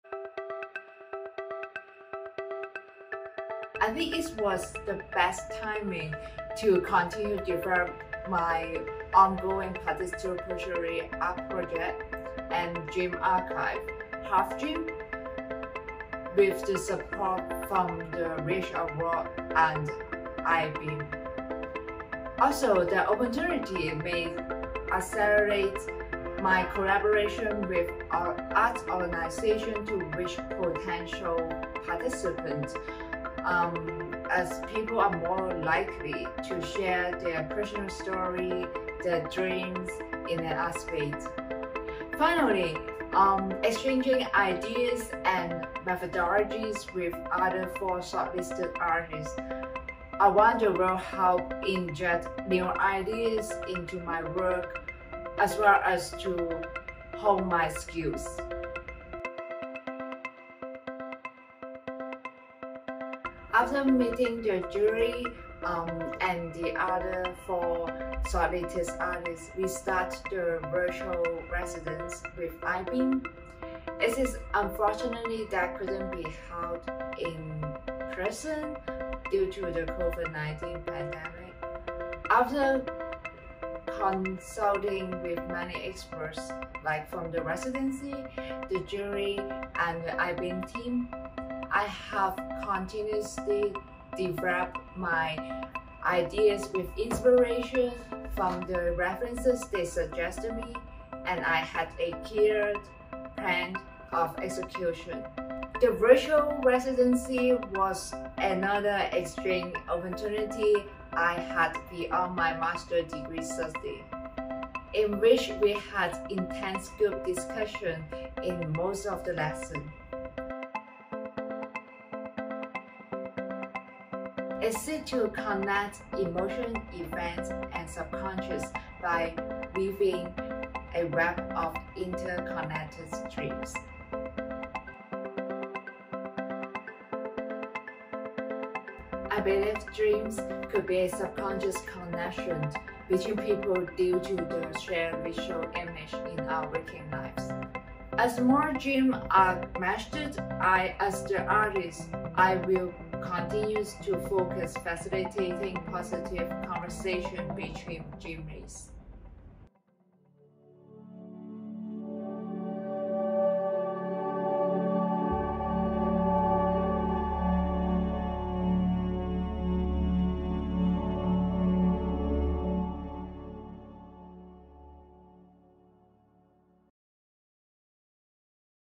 THE 4TH VH AWARD: Artist Interview - Doreen Chan
THE 4TH VH AWARD: Artist Interview

FINALIST - DOREEN CHAN, HalfDream

Doreen Chan, who was born in Hong Kong and now lives in Chicago, invites people to share their incomplete dreams through her platform halfdream.org. Chan then matches strangers based on the connections in their dreams, believing that something within the subconscious can help us see and remember the ways we are similar when external and political forces seem intent on manifesting division. As the sentences appear on the screen in Chan’s video work HalfDream (2021), there is an immediate sense of recognition—dreams of rushing, losing things, being in foreign places, alienated from oneself, or simply not oneself are perhaps more universal than we acknowledge.

홍콩에서 태어나 현재 시카고에 거주 중인 도린 챈은 그녀의 플랫폼 halfdream.org을 통해 사람들에게 그들의 불완전한 꿈을 공유하도록 초대합니다. 이어서, 챈은 꿈 사이의 연결성을 바탕으로 낯선 이들을 매칭하며, 외부 또는 정치 세력이 분열을 드러내는 와중에도 사실 잠재의식 속에서 우리는 모두 비슷하다는 것을 보고 기억하도록 합니다. 챈의 비디오 작품 "HalfDream" (2021) 속 등장하는 문장들을 보며 우리는  서두르는 꿈, 물건을 잃는 꿈, 외국에 있는 꿈, 자신에게서 소외되는 꿈 또는 단순히 자신이 아닌 꿈이 아마도 우리가 인식하는 것보다 더 보편적이라는 것을 즉각적으로 인식하게 됩니다.